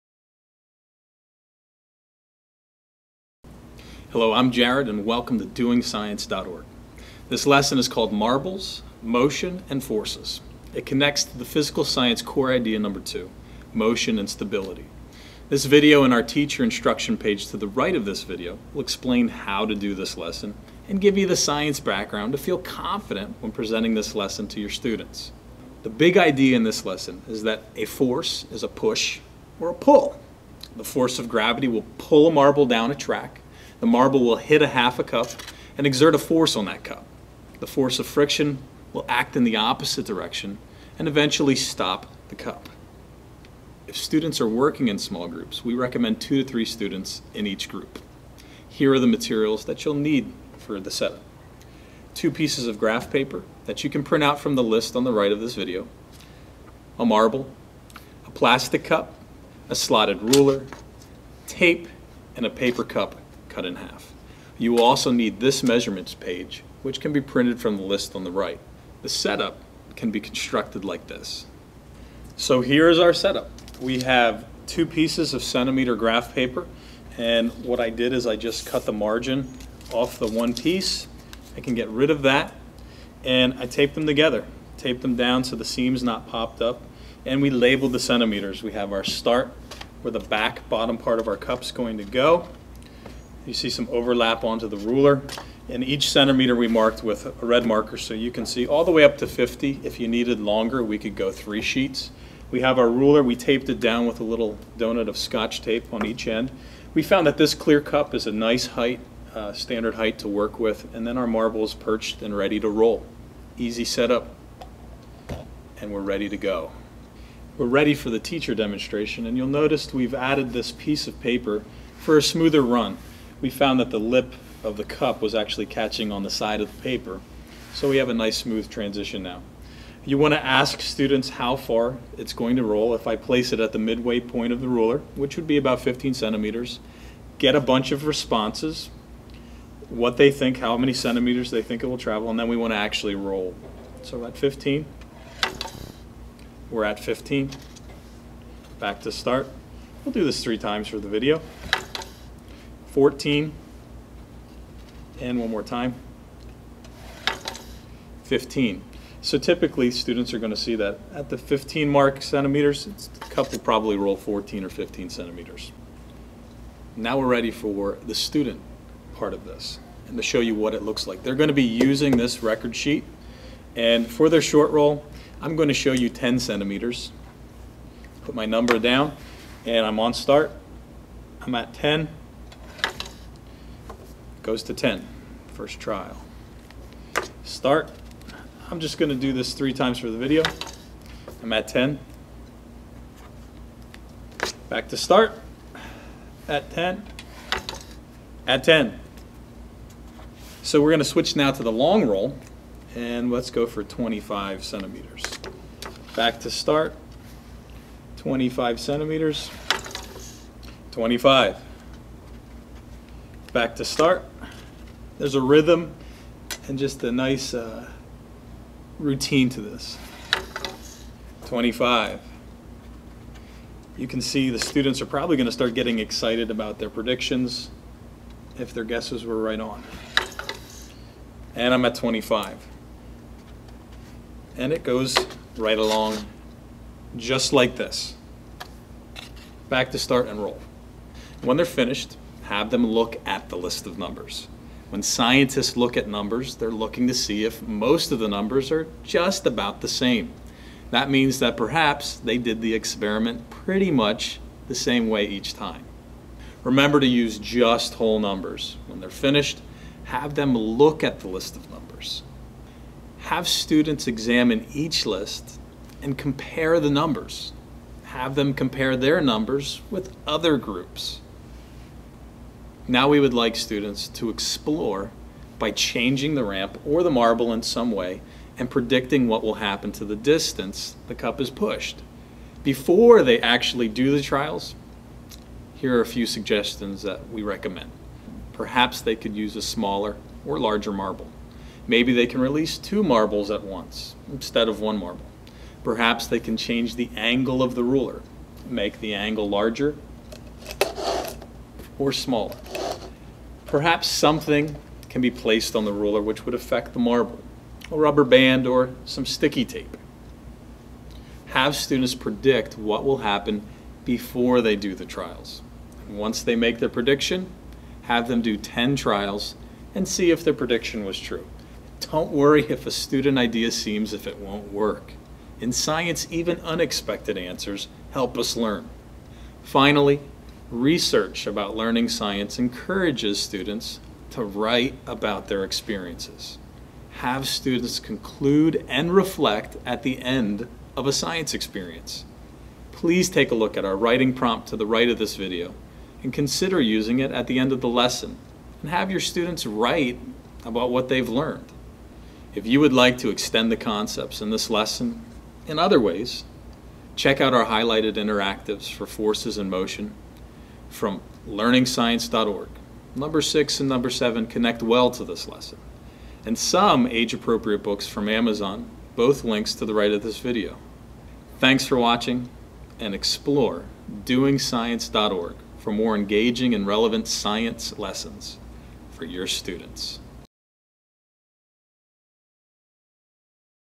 DoingScience.org Marbles Motion and Forces
A lesson to learn motion and forces for primary grade students